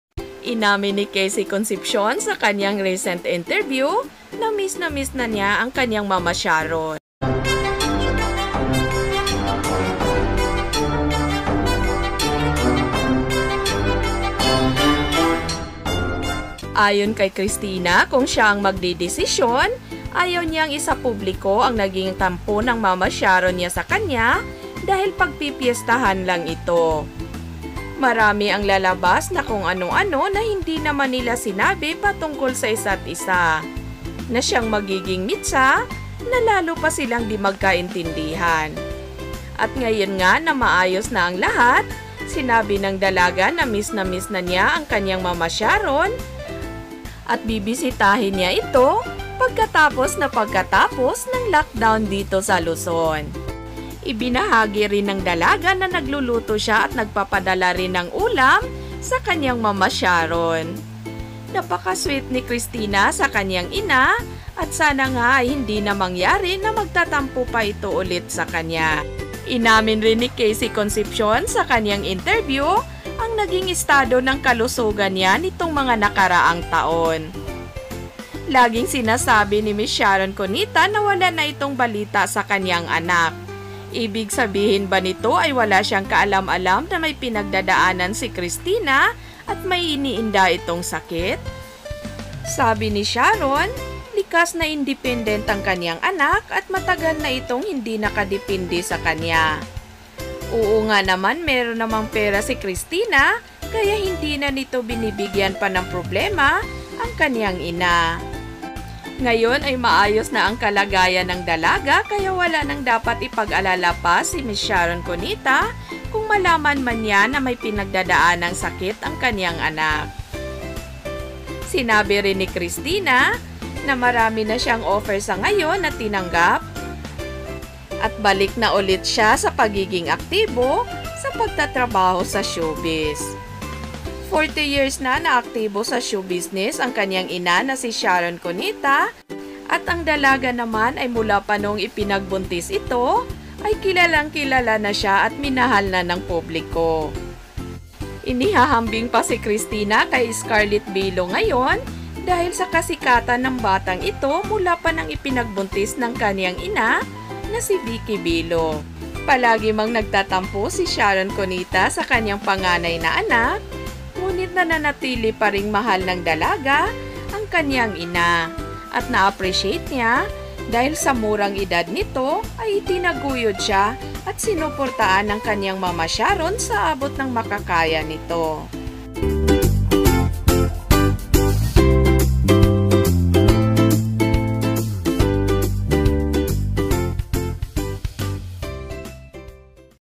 INAMIN NI KC CONCEPCION NA MISS NA MISS NA NYA ANG KANYANG INA NA SI SHARON CUNETA!
This is a showbiz vlog about kc concepcion and sharon cuneta!